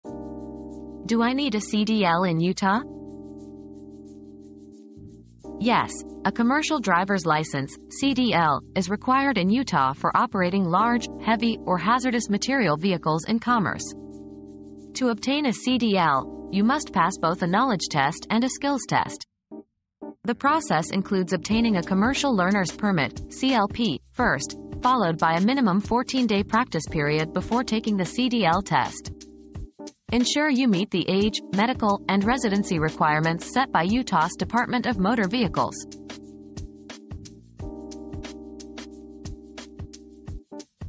Do I need a CDL in Utah?
CDL Requirements in Utah: Everything You Need to Know • Utah CDL Requirements • Discover if you need a CDL in Utah and learn about the process of obtaining one. Find out the age, medical, and residency requirements set by Utah's Department of Motor Vehicles and get ready to pass the knowledge and skills tests!